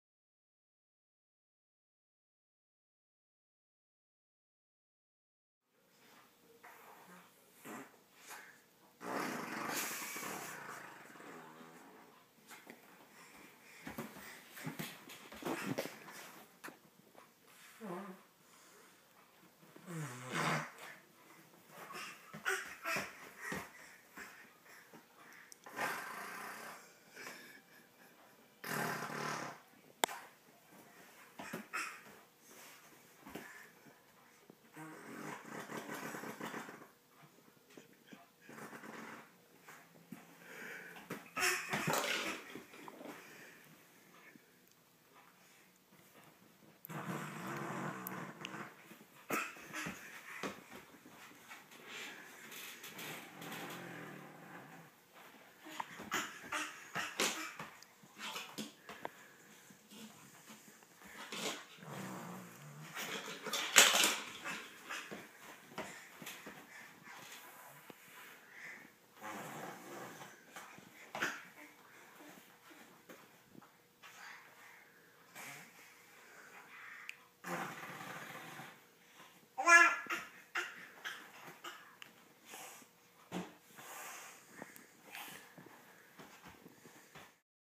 Funny Baby!
Kendall blowing bathroom noises on our leather sofa. Apparently making a game of it, too.